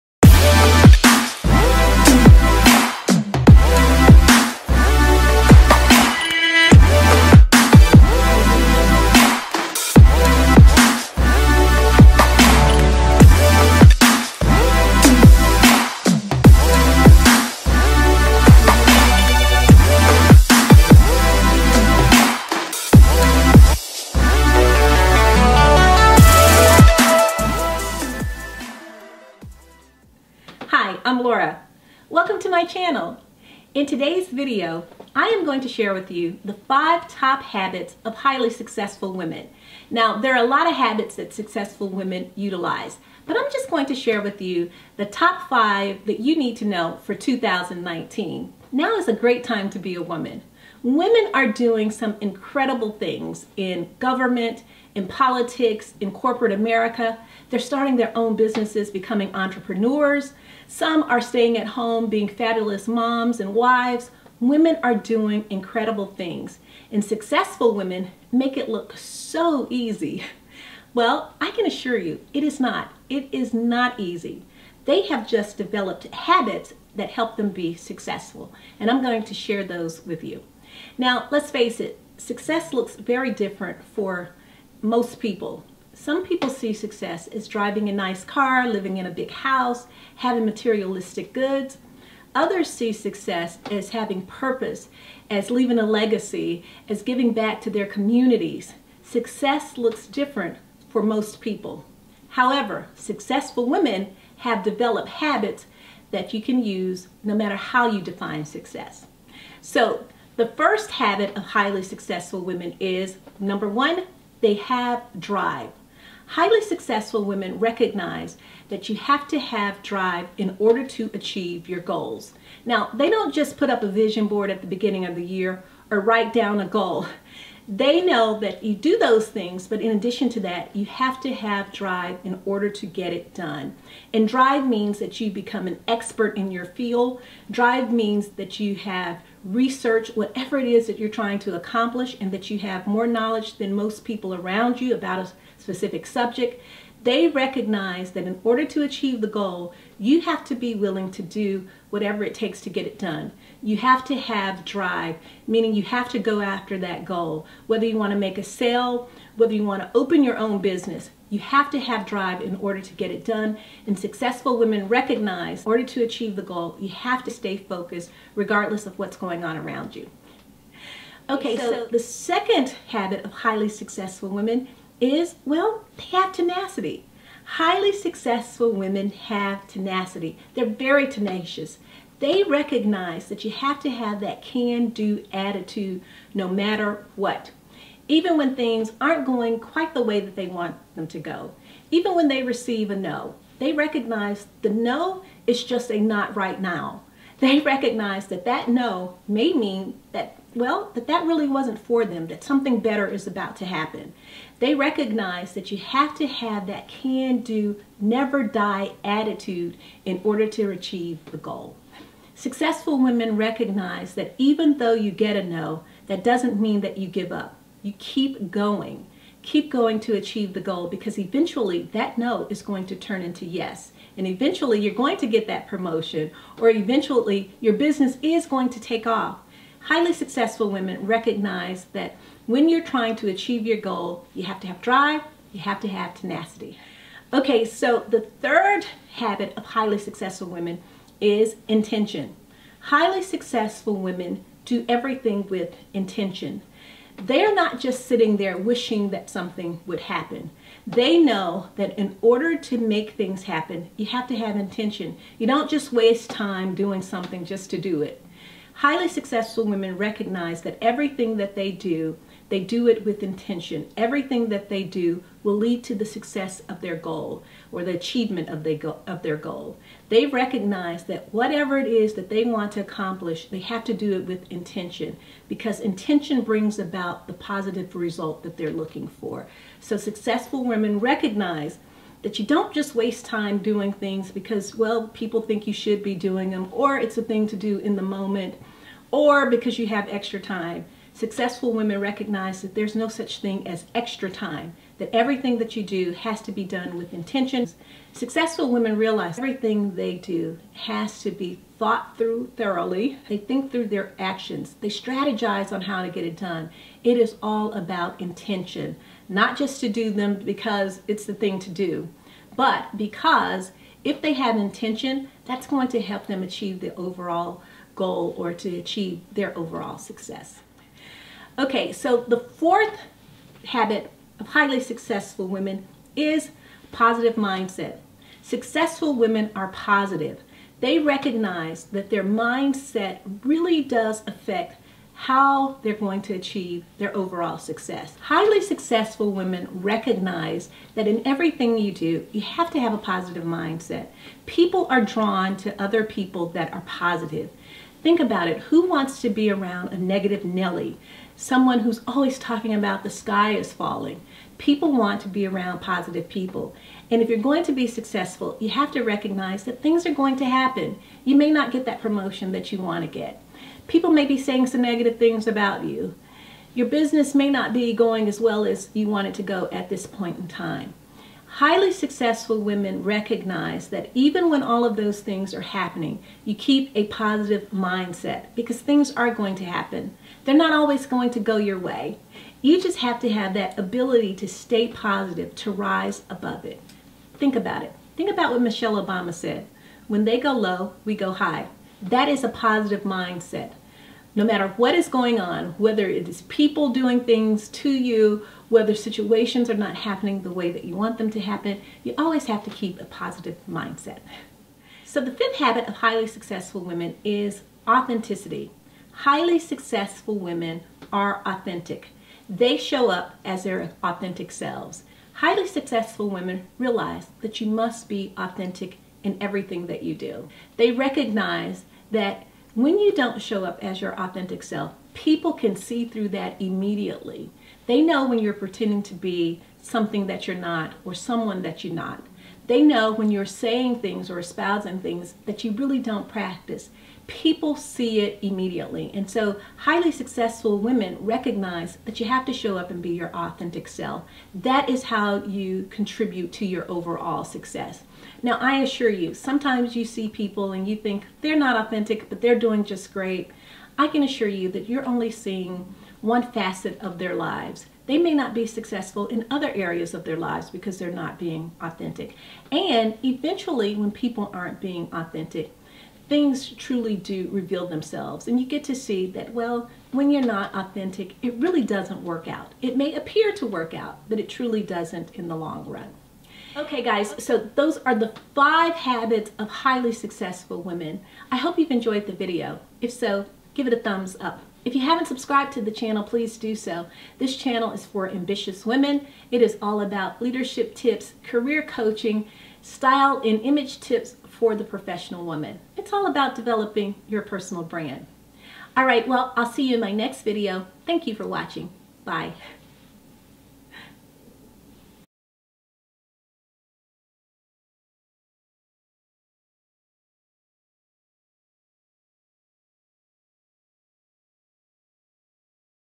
5 Habits of Highly Successful Women You Need to Know In 2019!
Hi Guys -  In this video I am sharing with you five habits of highly successful women. Some women make success look so effortlessly.  However, they only make it look that way. In actuality they have developed habits that help them achieve success. I hope you like the video.